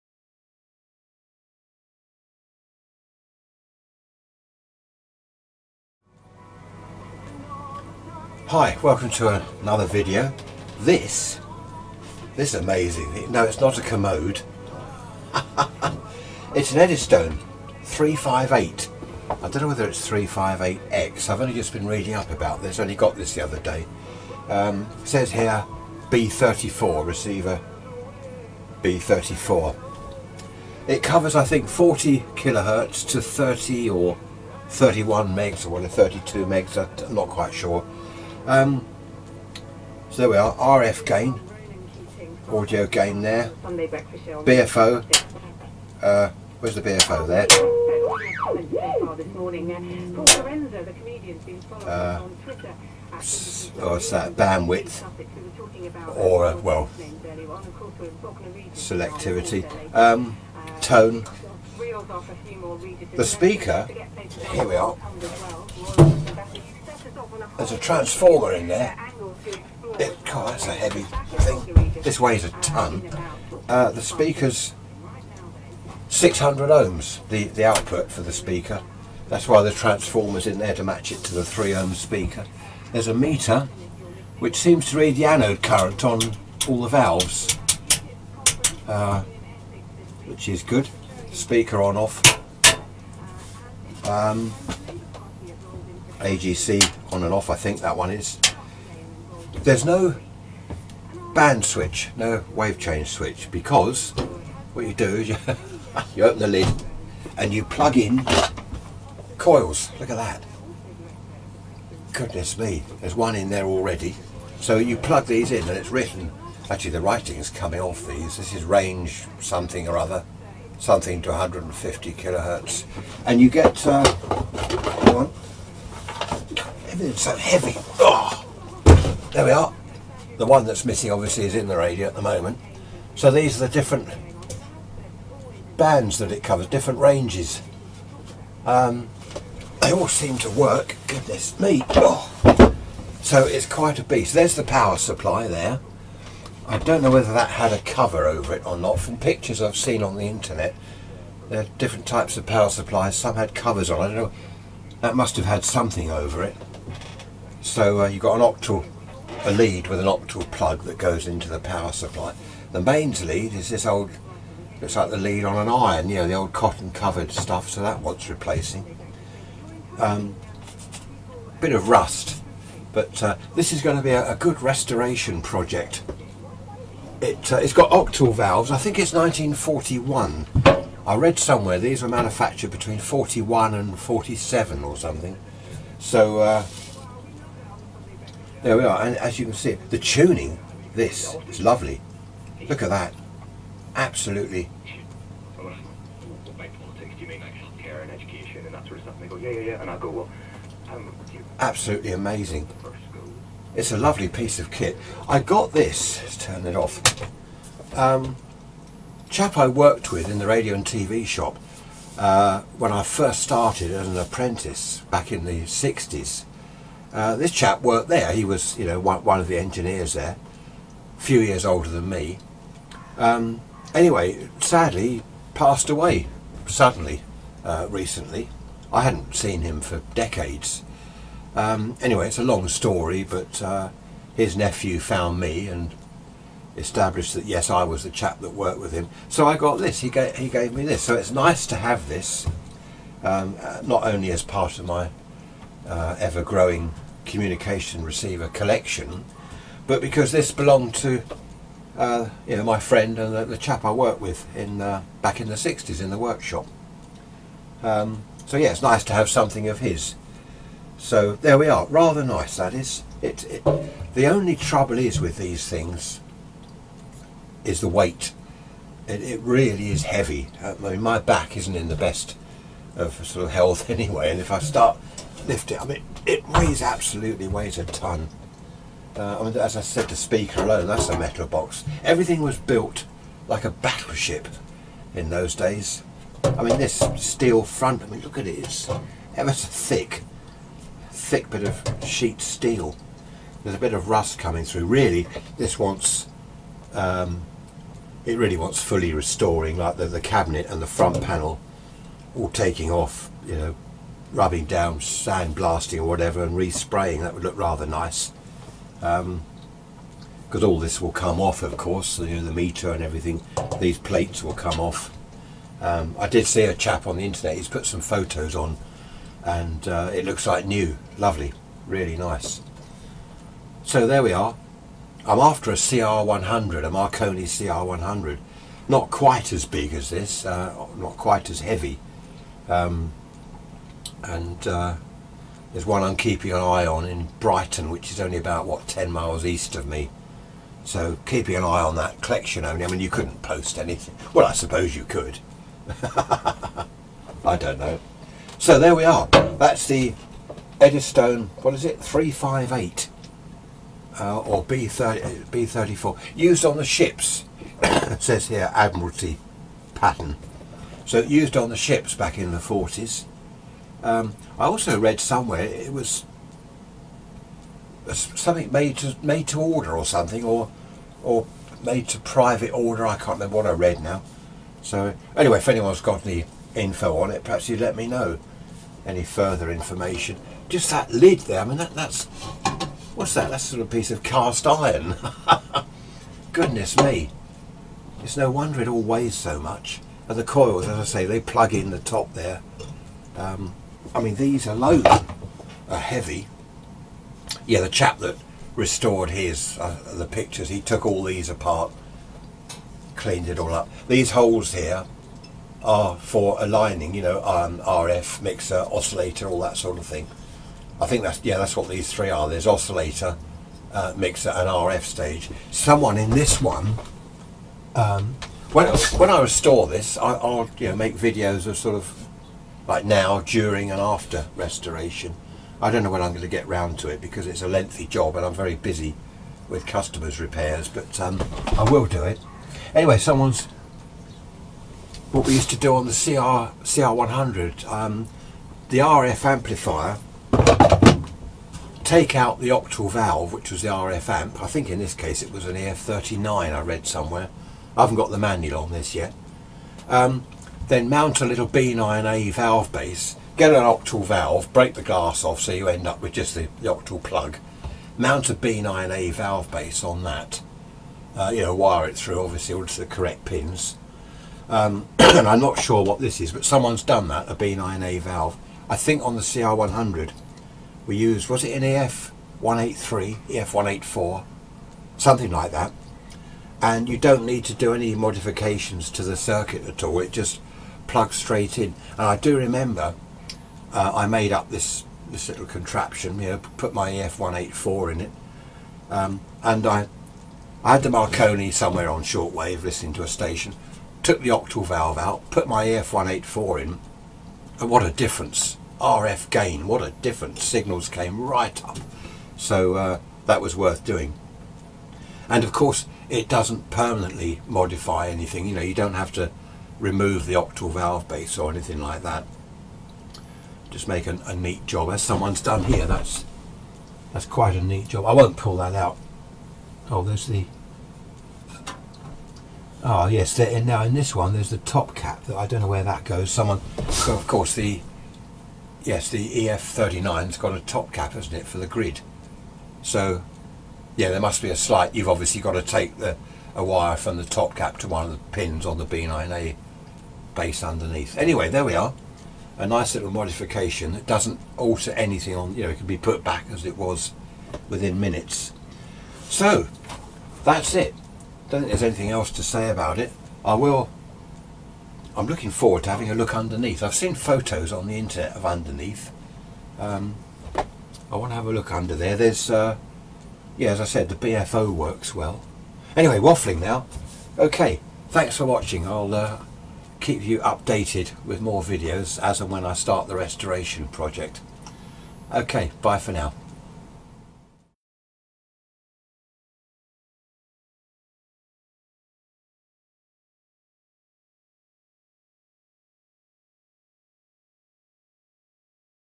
Eddystone 358X (B34) Communications Receiver - Part One
The Eddystone 358X (B34) Communications Receiver was first manufactured in 1941, as far as I can discover. It was designed for use aboard ships. Please look out for Part Two... coming soon.

You can comment on this video via Twitter... @Radioworkshop ... Or on my Facebook